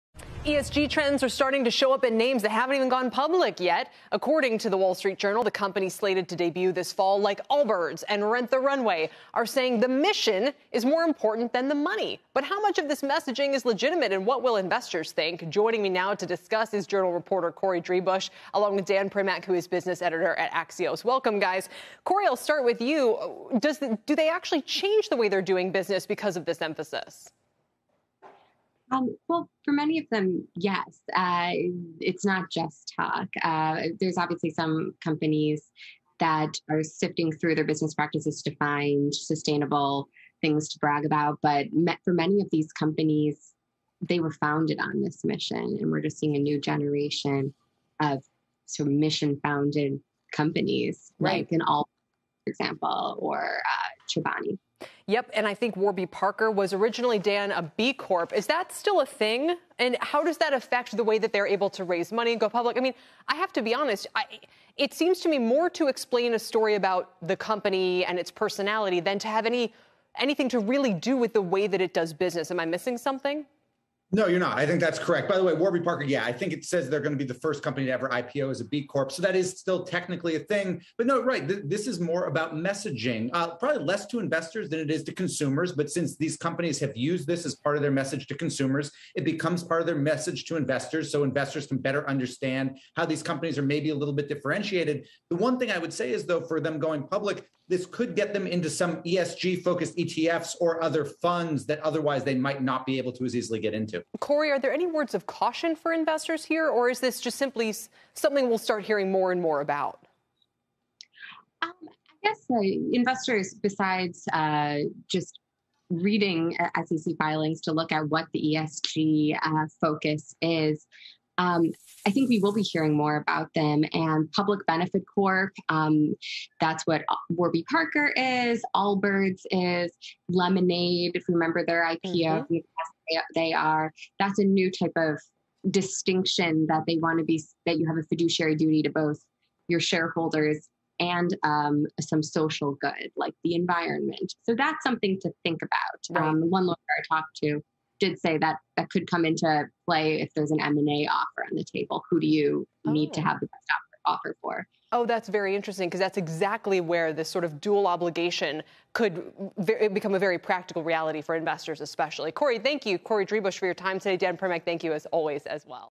Is mission more important than money in forthcoming IPOs?
Dan Primack, Axios' business editor, and Corrie Driebusch, a reporter for the Wall Street Journal, join The Exchange to discuss this season's upcoming IPOs and how companies like Allbirds, Sweetgreen and other will market themselves to investors.